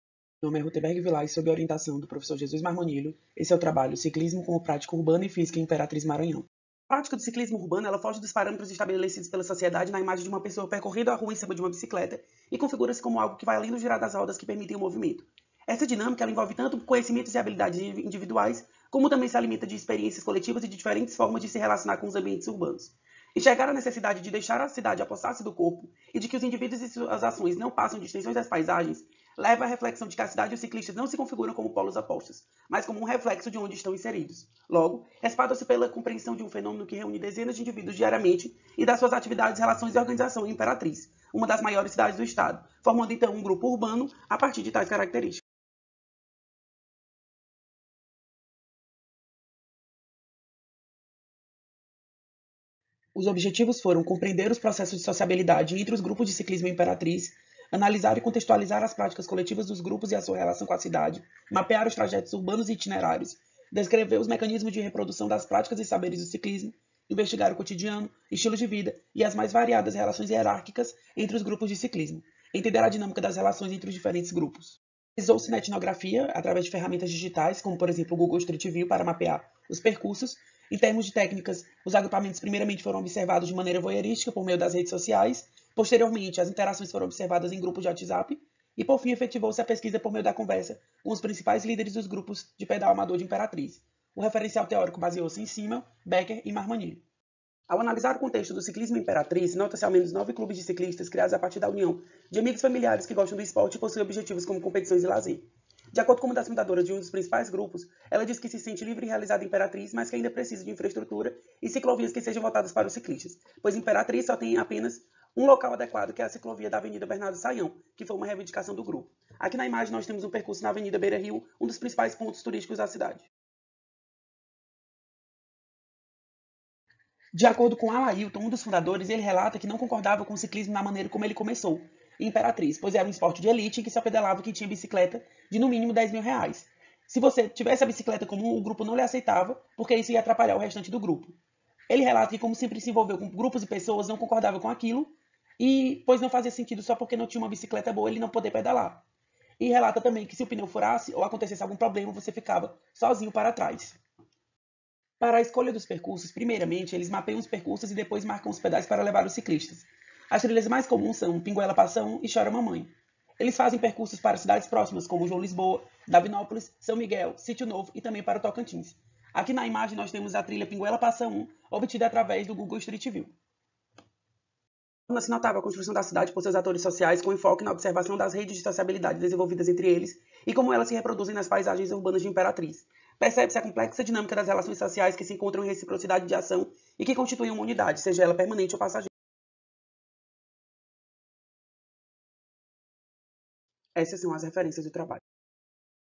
HU164 RUTEMBERG VILAR DE CARVALHO JUNIOR
"Titulo: O CICLISMO COMO PRÁTICA URBANA E FÍSICA EM IMPERATRIZ-MA

Autor(a): RUTEMBERG VILAR DE CARVALHO JUNIOR

Resumo: A dinâmica que envolve o ciclismo urbano envolve tanto conhecimentos e habilidades individuais, como também se alimenta de experiências coletivas e de diferentes formas de se relacionar com os ambientes urbanos. O presente plano de trabalho visa abordar as relações sociais aparentes e implícitas do ciclismo como prática urbana e física em Imperatriz-MA. Para além do pedalar, ressalta-se a necessidade de compreender como esses indivíduos se organizam nos devidos grupos, qual as relações sociais desempenhadas por eles, e como se dão os processos de conhecimento e relação com a cidade, itinerários e rotas. Objetivando o conhecimento das relações e do cotidiano, utilizou-se de método netnográfico. Posteriormente, as interações foram observadas em grupos de Whatsapp. Por fim, efetuou-se a pesquisa por meio da conversa com os principais líderes dos grupos de pedal amador da Imperatriz. A análise foi orientada pelos conceitos de Georg Simmel e suas contribuições em relação aos agrupamentos, formações de unidade e os efeitos da sociabilidade na formação de unidades. Amparou-se ainda o pensamento de Howard Becker a partir de compreender como os processos de sociabilidades estão determinados dentro das representações, e como essas expressam a relação desses indivíduos com a cidade. Ademais, utilizou-se do Google Street View na pesquisa etnográfica para mapear os trajetos urbanos. Tornou-se notável a construção da cidade por seus atores sociais, com enfoque na observação das redes de sociabilidade desenvolvidas entre eles e como elas se reproduzem nas paisagens urbanas. O uso da bicicleta como uma maneira de habitar e produzir a cidade na medida em que ocupa esses espaços, está diretamente ligado a interpretação das práticas e das relações sociais como peças importantes na percepção que se tem do ambiente e das paisagens urbanas. Logo, a ação de pedalar configura-se como uma maneira específica de viver a cidade e de contribuir para a construção do ambiente urbano. Palavras-chave: Ciclismo; Cidade; Movimento.